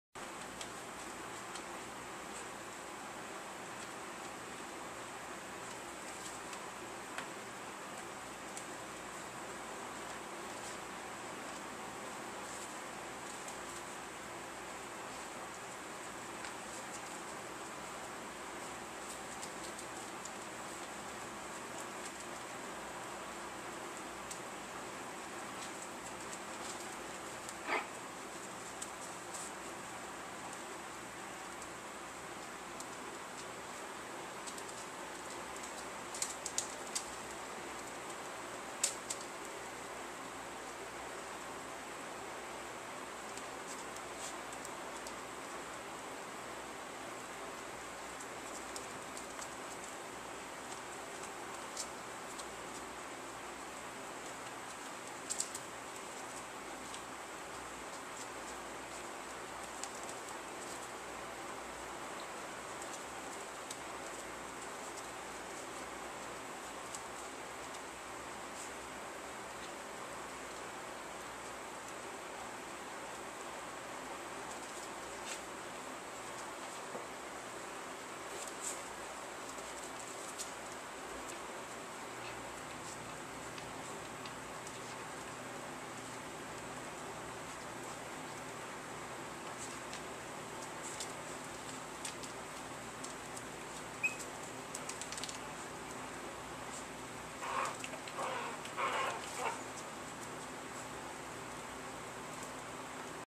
Chihuahua puppies, Grand Champion sired puppies @ almost 8 wk of age
This video was taken on the 7th of September 2011; they are all near 8 weeks of age.

The youngest puppy is Baylee; she is a total fuzz ball. She is full of energy and very people friendly. She is a very promising show prospect; she should mature in the mid 5lb range, which will be a safe breeding size. She is still available for sale for @ $1,250.00.

The long-coat black puppy is Thor. He is calm and easy to handle. He is a nice show prospect; he should mature in the low to mid 4lb range, which will be an ideal breeding size. He is still available for sale for @ $ 750.00.

The short coat spotted (more black) puppy is Cara, she is available for sale, email for info. The other spotted (more white) puppy and the tiny brown puppy are sold.